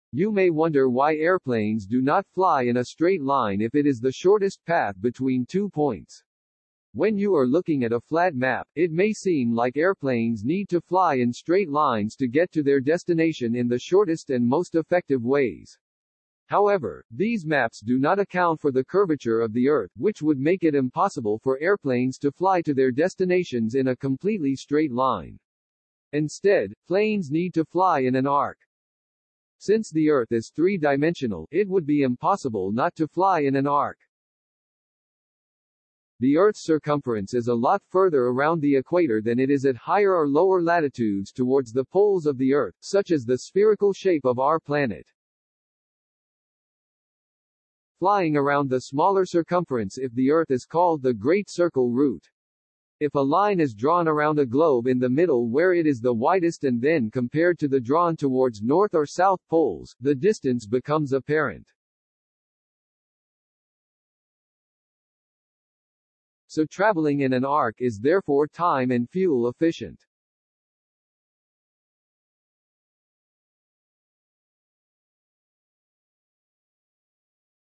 Why Aeroplanes don't travel in a straight line | Curved Path of Flights | Get to Know
Get to know the reason behind curved path of aeroplanes as seen through a map!